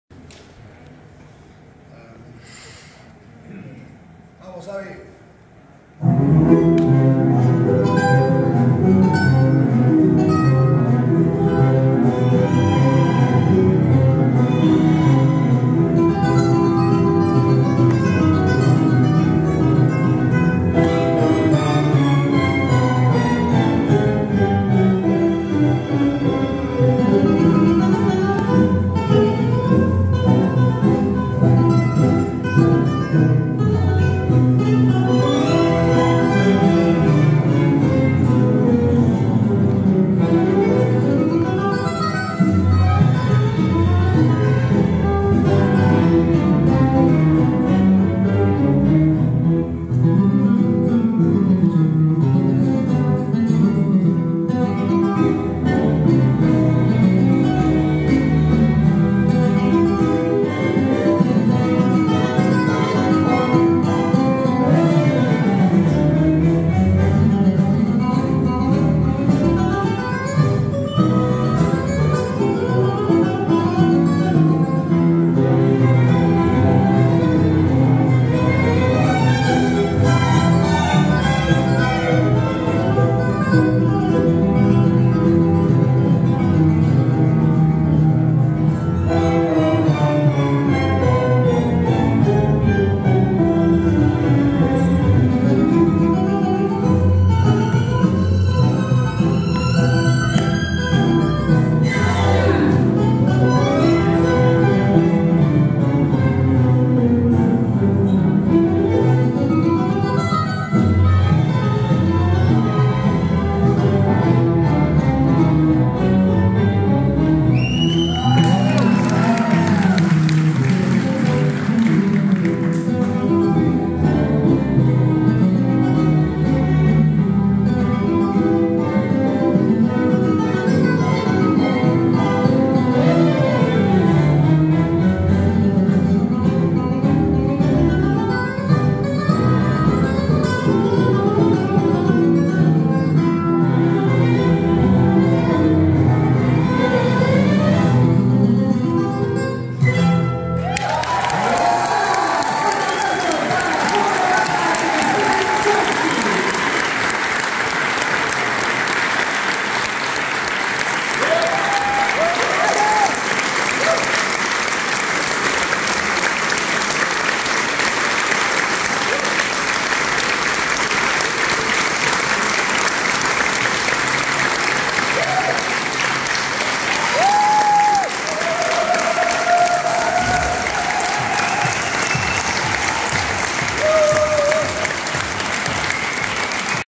Joe Corbata e Lucila Cionci no Milongueros All Aboard em 25/11/22. 2/4
Na Herança Magna, Vila Nova de Gaia, Porto, Portugal.